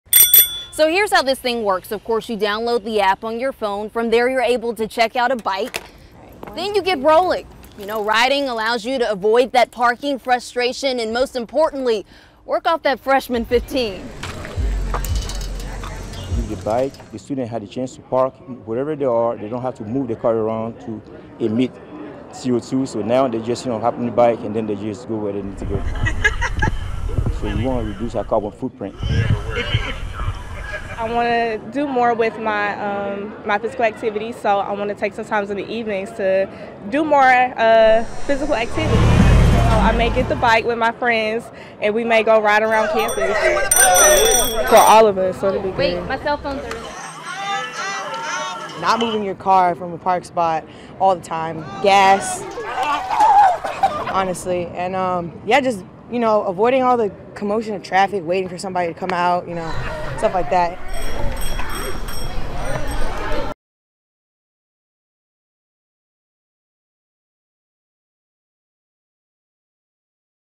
Fayetteville State University rolls out new bike service
Fayetteville State University is rolling out it's Lime Bike service.